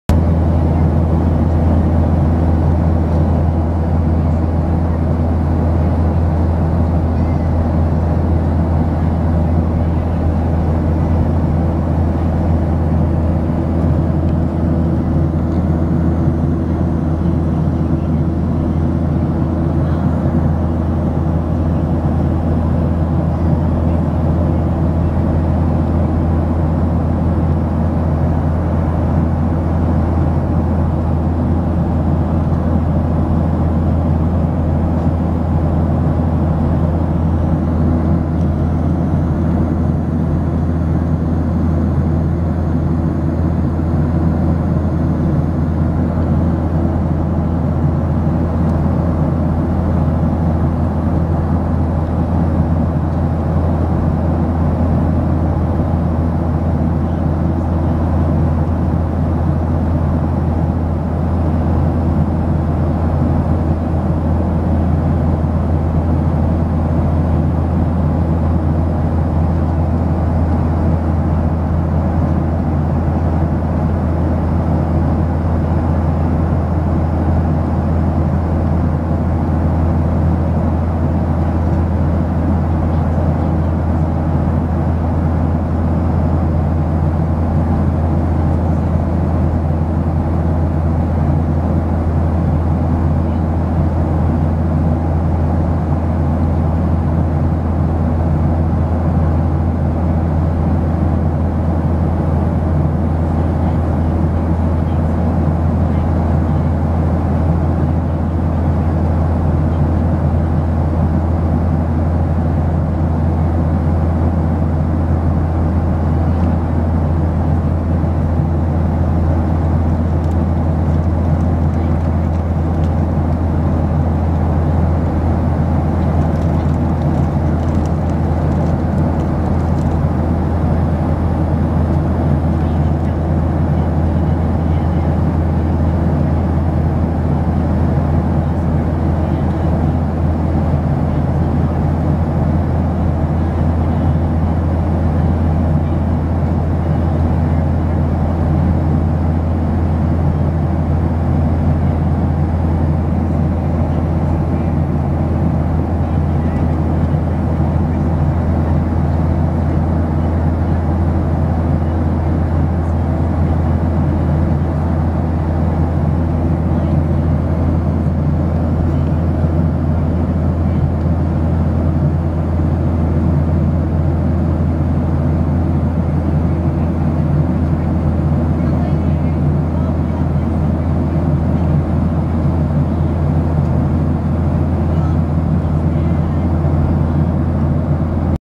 Pretty Fluffy Clouds From the Southwest Plane
About 15,000 feet I think